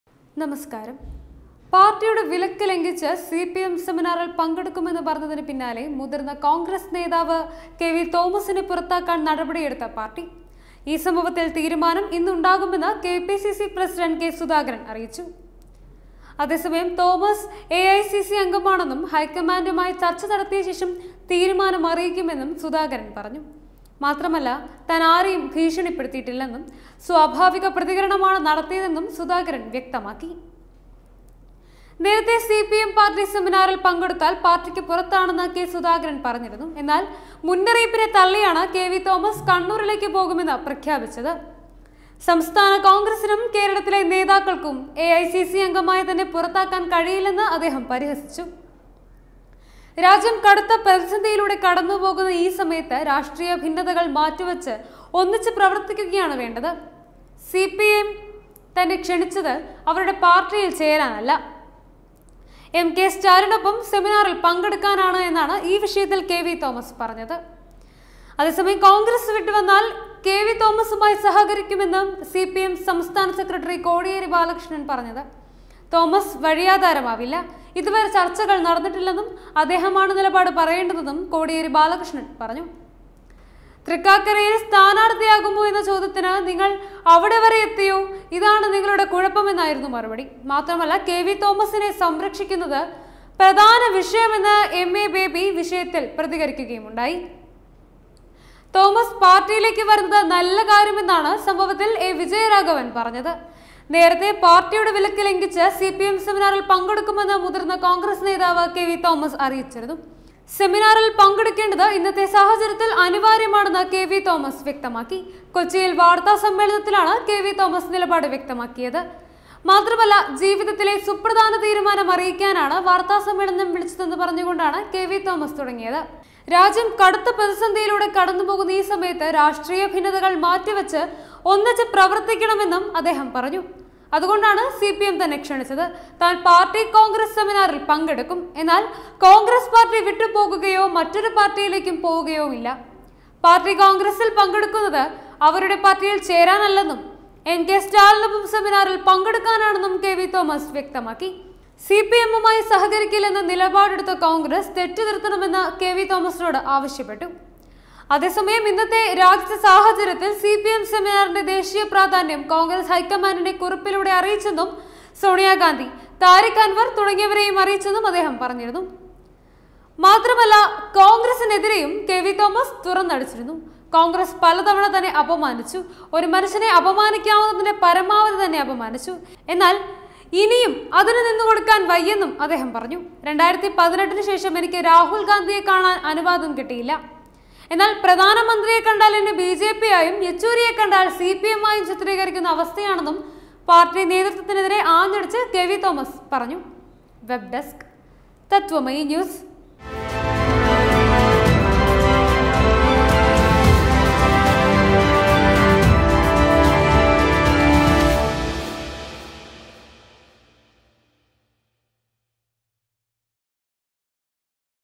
പുകഞ്ഞ കൊള്ളി പുറത്തേക്കെന്ന് കെ പി സി സി ? | K V THOMAS
കെ വി തോമസ് ഇനി മുതൽ സഖാവ് കെ വി തോമസ് ? | K V THOMAS